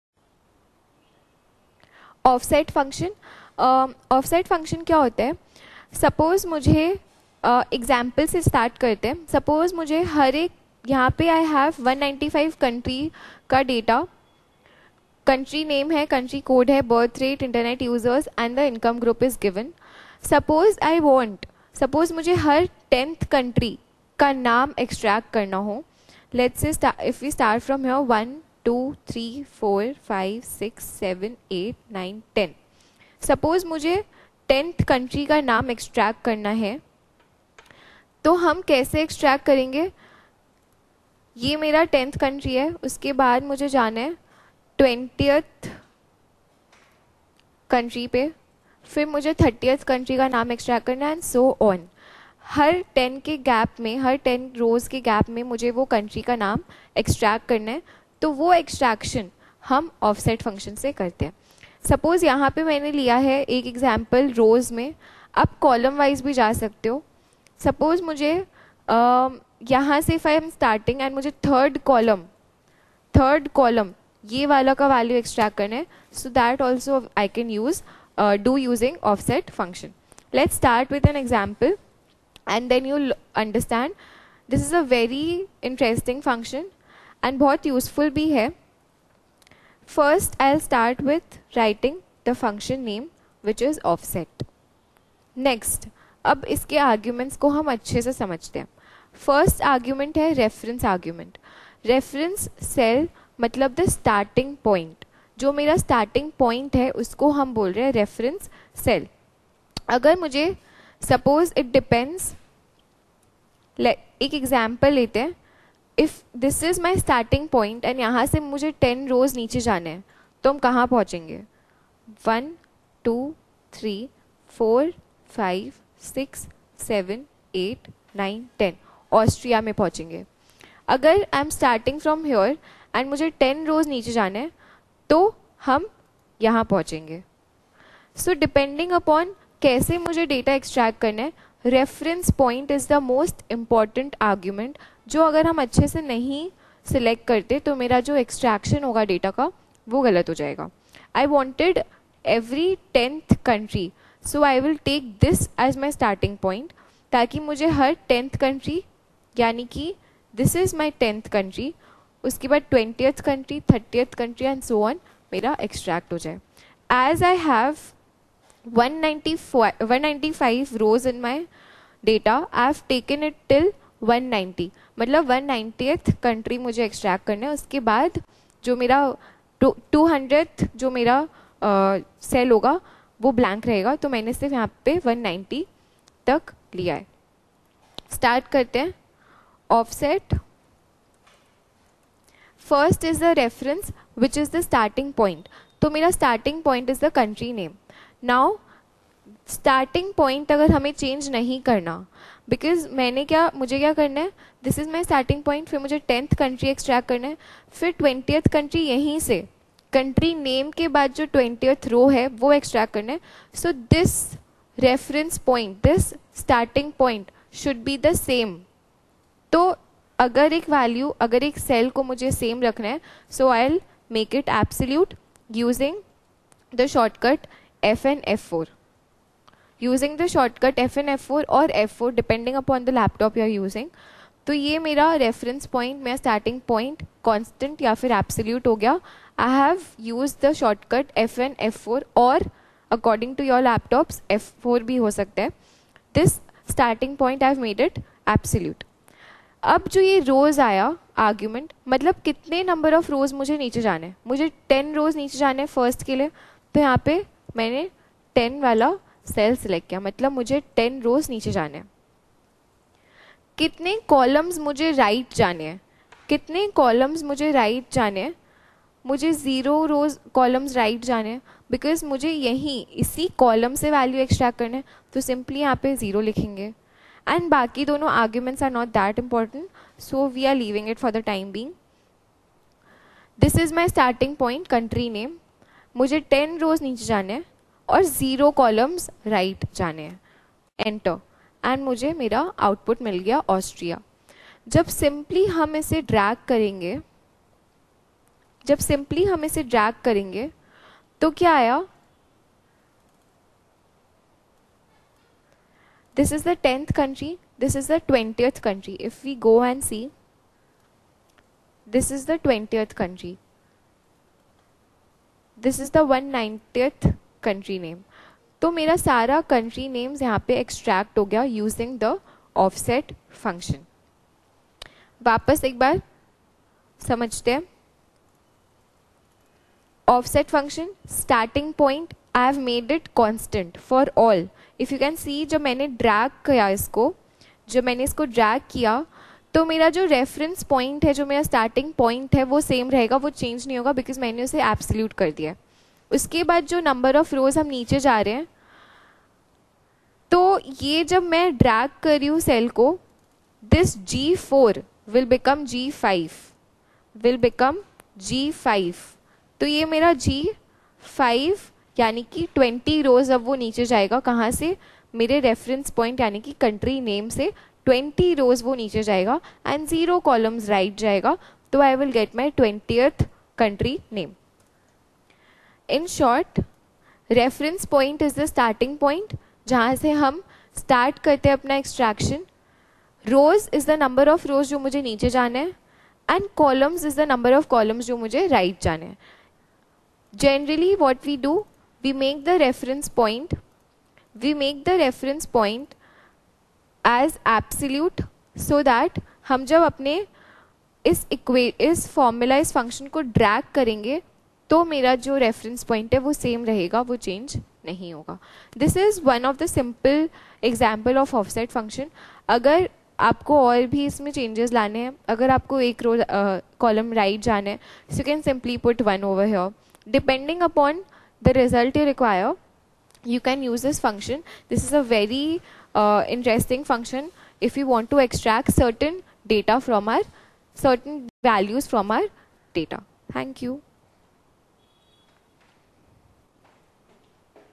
30 Days of Excel | Offset fuction | Shivangee Agarwal Ma'am | AEI
Day 13 of 30 Days of Excel Challenge: Master the Offset function! Navigate through your data dynamically and access what you need with precision.